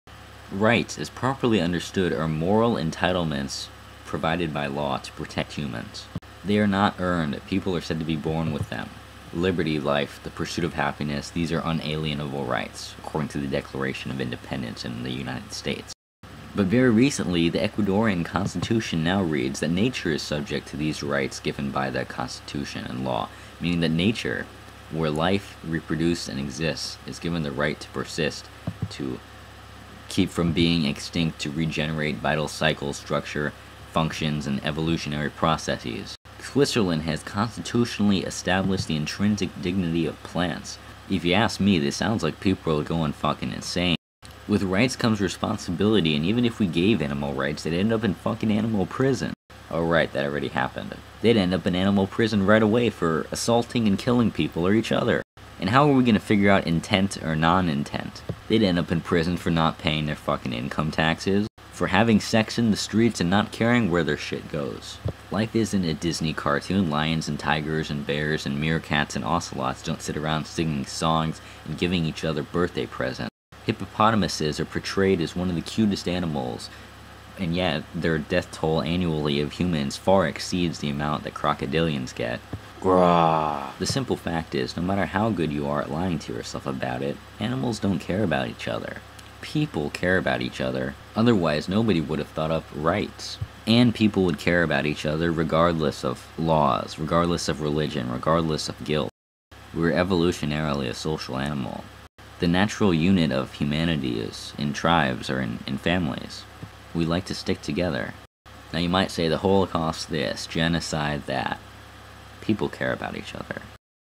Animal Rights Constitutionally Established
In Ecuador and Switzerland the rights of "nature" have been Constitutionally represented and established. What does this mean?

And forget the issue that this is unenforceable. Perhaps to hunters, but what when animals commit the crimes against nature? Animals can't because they are part of nature. And so are we.

thecast, now in widescreen! :D